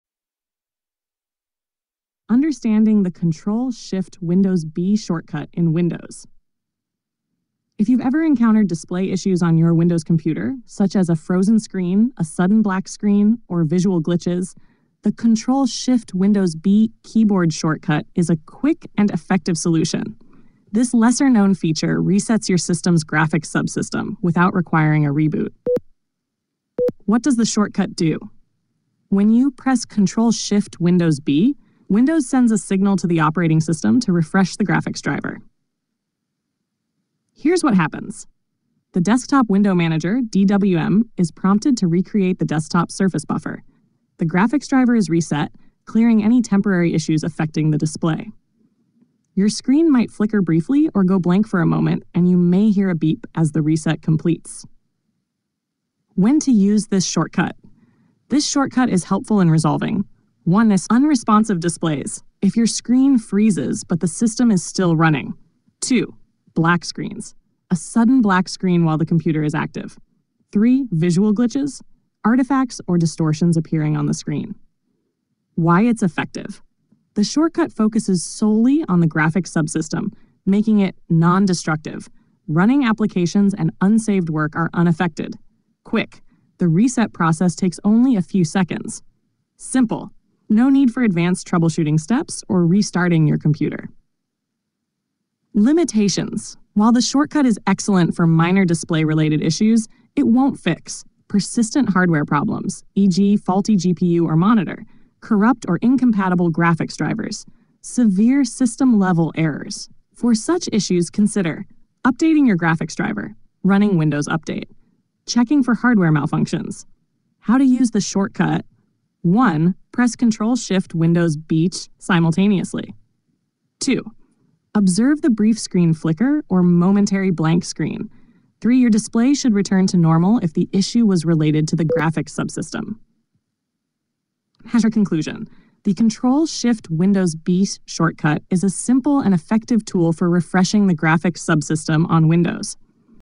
Reset Graphics Driver with Ctrl + Shift + Windows + B on Windows
Learn how to use the Ctrl + Shift + Windows + B shortcut to fix frozen screens, black displays, or glitches in Windows by resetting the graphics driver instantly.